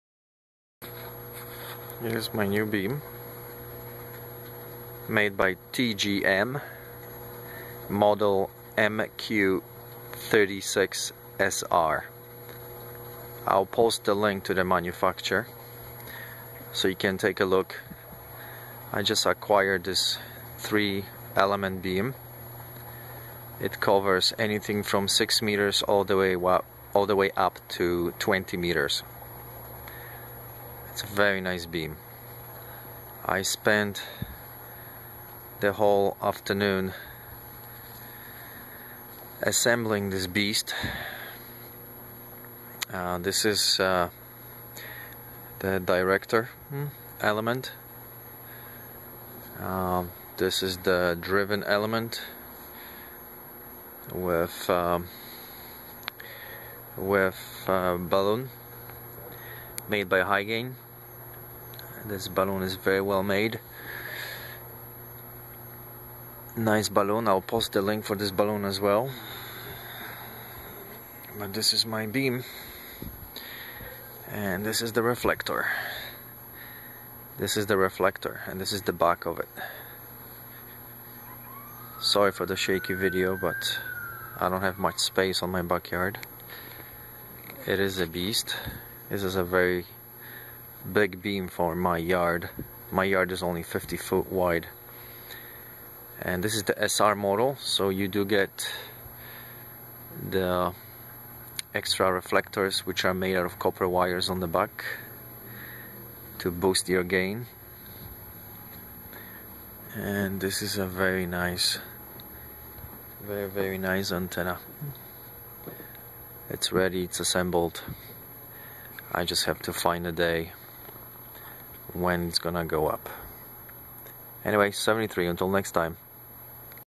MQ-36SR HF Beam Antenna
My new HF beam MQ36SR which is going to replace my MQ26 HF beam.